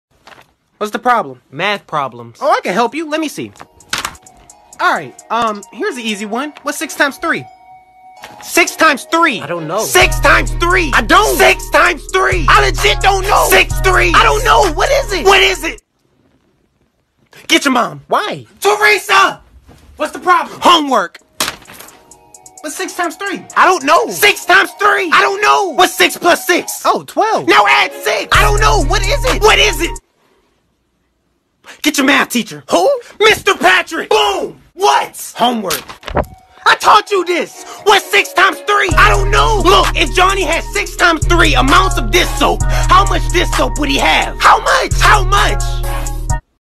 ･:,｡★ﾟ○.::【MMD ♡ Friends ♡ OC】::. ◤♡ When no one knows what 6 x 3 equals ♡◢ ::.○･:,｡★ﾟ
.:: Créditos♡ :

♡ Motion by Lua͟n̴à Animations

ﾟ･:,｡ﾟ･:,｡★ﾟ･:,｡ﾟ･:,｡☆ﾟ･:,｡ﾟ･:,｡★ﾟ･:,｡ﾟ･:,｡☆ﾟ･:,｡ﾟ･:,｡★ﾟ･:,｡ﾟ･

.:: Modelos♡ :

○ MMD- Cute -Cookie by me
DL : only friends

○ Misaki by ღMɪsᴀᴋɪ-sᴀɴ†
DL : Nop

○ Yeye :3 by Yeye :3
DL : Nop

○ Nightcore Vaira uwu by Natsuki7w7r
DL : Nop

ﾟ･:,｡ﾟ･:,｡★ﾟ･:,｡ﾟ･:,｡☆ﾟ･:,｡ﾟ･:,｡★ﾟ･:,｡ﾟ･:,｡☆ﾟ･:,｡ﾟ･:,｡★ﾟ･:,｡ﾟ･

.:: Stage :

by ???

ﾟ･:,｡ﾟ･:,｡★ﾟ･:,｡ﾟ･:,｡☆ﾟ･:,｡ﾟ･:,｡★ﾟ･:,｡ﾟ･:,｡☆ﾟ･:,｡ﾟ･:,｡★ﾟ･:,｡ﾟ･

♡ bye bye ʕっ•ᴥ•ʔっ ♡